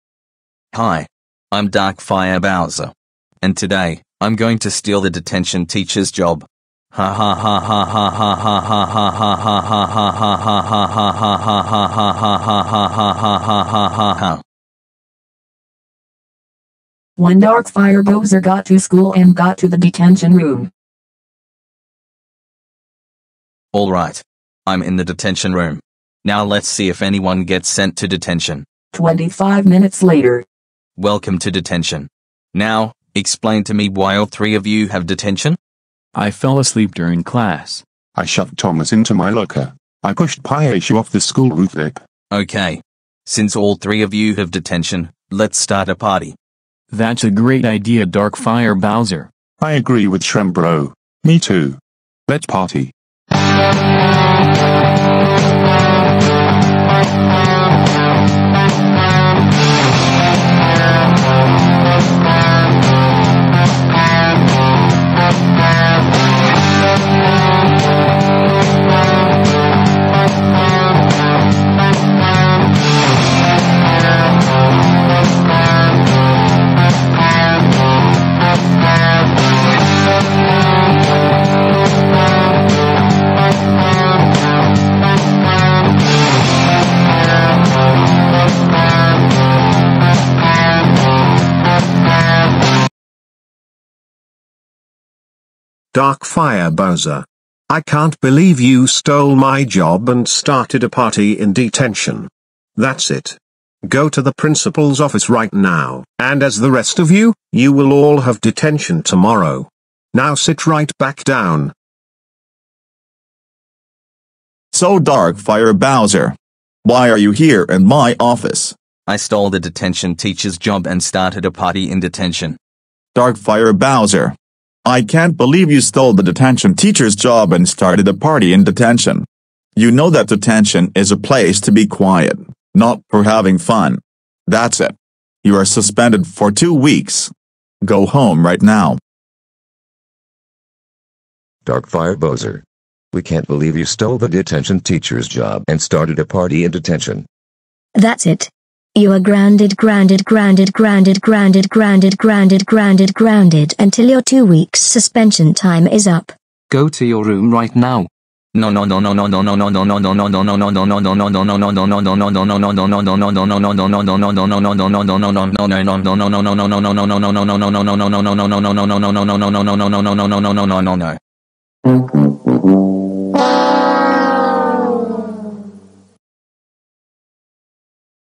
Dark Fire Bowser steals the detention teacher's job/Grounded
Inspired by: @ThomasTheNorthwesternVA, @blackholetheking2781, @ethantheultimatekingdomhea1455, @YavorCh2005, @mrsuperinc41-gd4ub, and @superromanreignsnick7203!